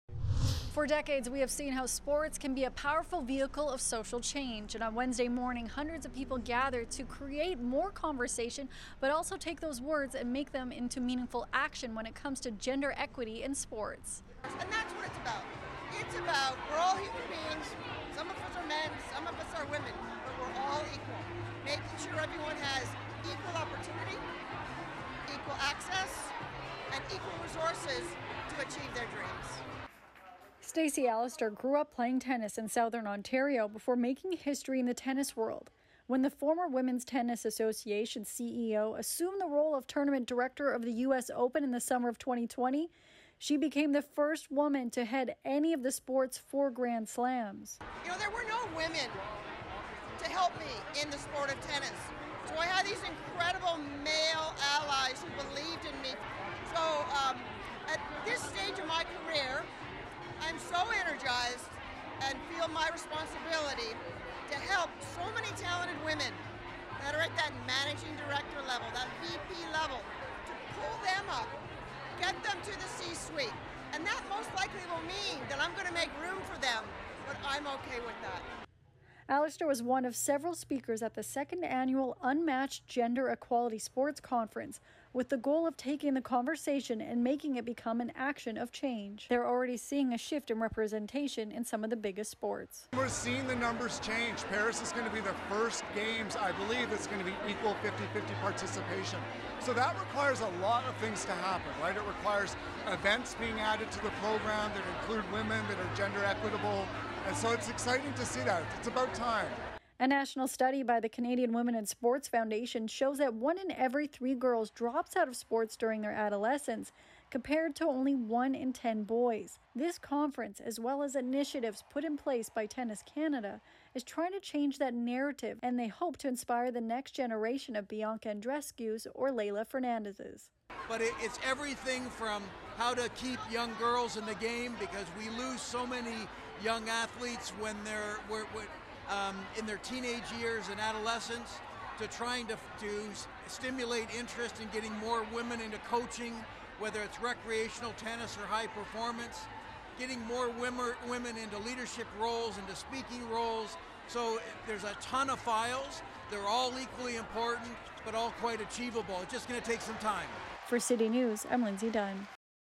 Game. Set. Equity. A shift in sports representation
Lindsay Dunn speaks to members of Canada's Olympic committee as well the US Open and Tennis Canada about the shift towards women in sports.